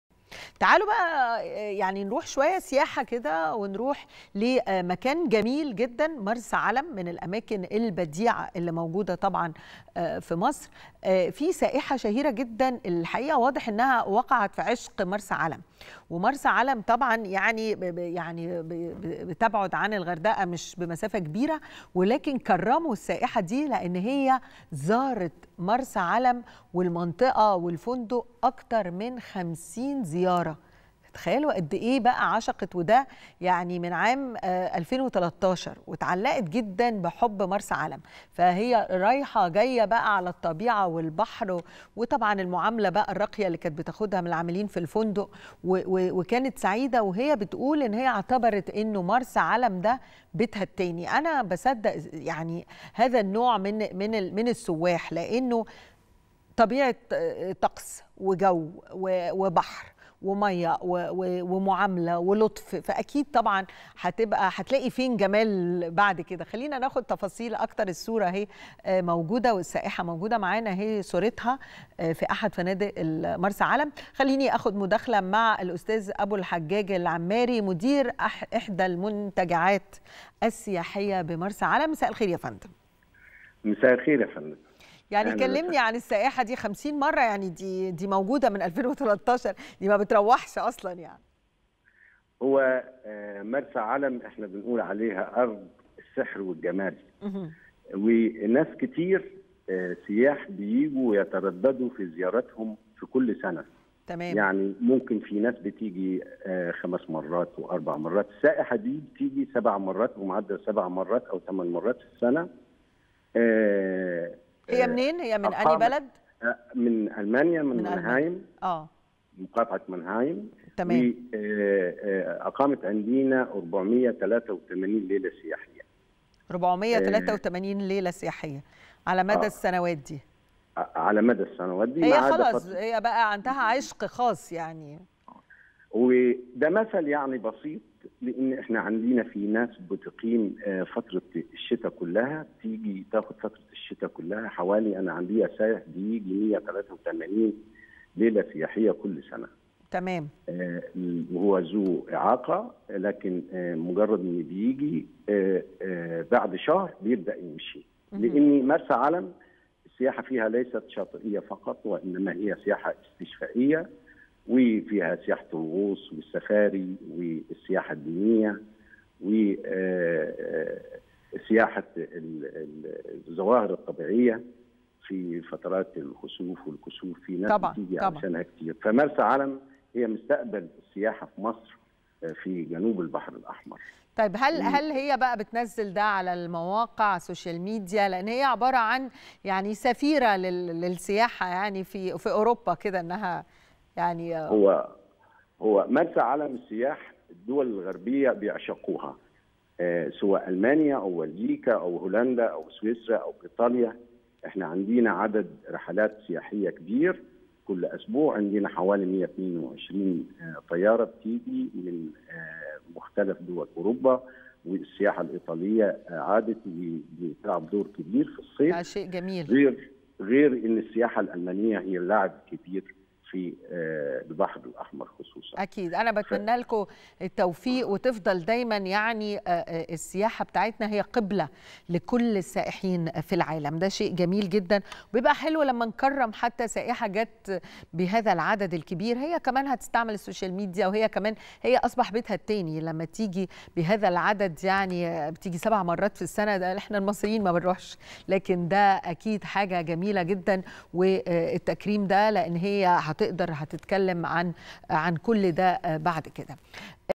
تكريم سائحة ألمانية تعشق مرسى علم.. أبو الحجاج العماري مدير أحد المنتجعات السياحية يوضح التفاصيل
التاسعة | تكريم سائحة ألمانية تعشق مرسى علم.. أبو الحجاج العماري مدير أحد المنتجعات السياحية يوضح التفاصيل